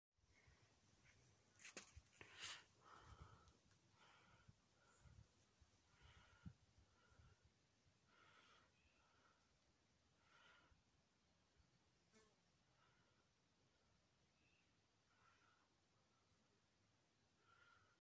ginger cat loaf
This is chesho, my grandma's ginger cat.